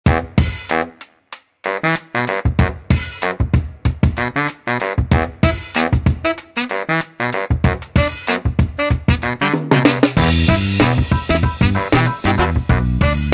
Siebenpunkt-Marienkäfer (Coccinella septempunctata)
wilder Marienkäferbock bei der Paarung